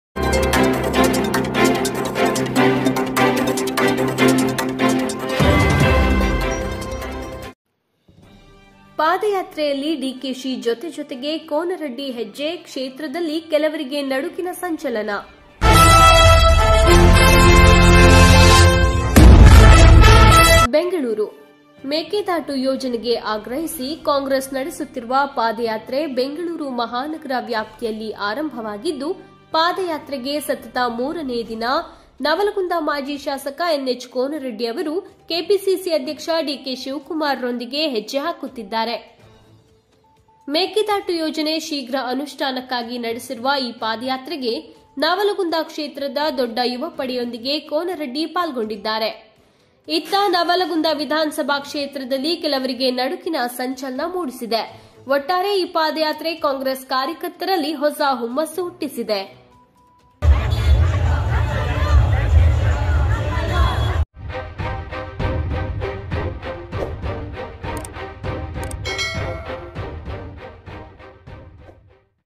ಡಿಕೆಶಿ ಜೊತೆ ಕೋನರಡ್ಡಿ ಹೆಜ್ಜೆ,ಕ್ಷೇತ್ರದಲ್ಲಿ ಕೆಲವರಿಗೆ ನಡುಕಿನ ಸಂಚಲನ
ಡಿಕೆಶಿ ಜೊತೆ ಕೋನರಡ್ಡಿ ಹೆಜ್ಜೆ, ಕೆಲವರಿಗೆ ನಡುಕಿನ ಸಂಚಲನ.

ಬೆಂಗಳೂರು : ಮೇಕೆದಾಟು ಯೋಜನೆಗೆ ಆಗ್ರಹಿಸಿ ಕಾಂಗ್ರೆಸ್ ನಡೆಸುತ್ತಿರುವ ಪಾದಯಾತ್ರೆ ಬೆಂಗಳೂರು ಮಹಾನಗರ ವ್ಯಾಪ್ತಿಯಲ್ಲಿ ಆರಂಭವಾಗಿದ್ದು,  ಪಾದಯಾತ್ರೆಗೆ ಸತತ ಮೂರನೇ ದಿನ ನವಲಗುಂದ ಮಾಜಿ ಶಾಸಕ ಎನ್ ಎಚ್ ಕೋನರಡ್ಡಿಯವರು ಕೆಪಿಸಿಸಿ ಅಧ್ಯಕ್ಷ ಡಿಕೆ ಶಿವಕುಮಾರ್ ನೊಂದಿಗೆ ಹೆಜ್ಜೆಹಾಕುತ್ತಿದ್ದಾರೆ.

ಮೇಕೆದಾಟು ಯೋಜನೆ ಶೀಘ್ರ ಅನುಷ್ಠಾನಕ್ಕಾಗಿ ನಡೆಸಿರುವ ಈ ಪಾದಯಾತ್ರೆಗೆ ನವಲಗುಂದ ಕ್ಷೇತ್ರದ ದೊಡ್ಡ ಯುವ ಪಡೆಯೊಂದಿಗೆ ಕೋನರಡ್ಡಿ ಪಾಲ್ಗೊಂಡಿದ್ದರೆ.

ಇತ್ತ ನವಲಗುಂದ ವಿಧಾನಸಭಾ ಕ್ಷೇತ್ರದಲ್ಲಿ ಕೆಲವರಿಗೆ ನಡುಕಿನ ಸಂಚಲನ ಮೂಡಿಸಿದೆ. ಒಟ್ಟಾರೆ ಈ ಪಾದಯಾತ್ರೆ ಕಾಂಗ್ರೆಸ್ ಕಾರ್ಯಕರ್ತರಲ್ಲಿ ಹೊಸ ಹುಮ್ಮಸ್ಸು ಹುಟ್ಟಿಸಿದೆ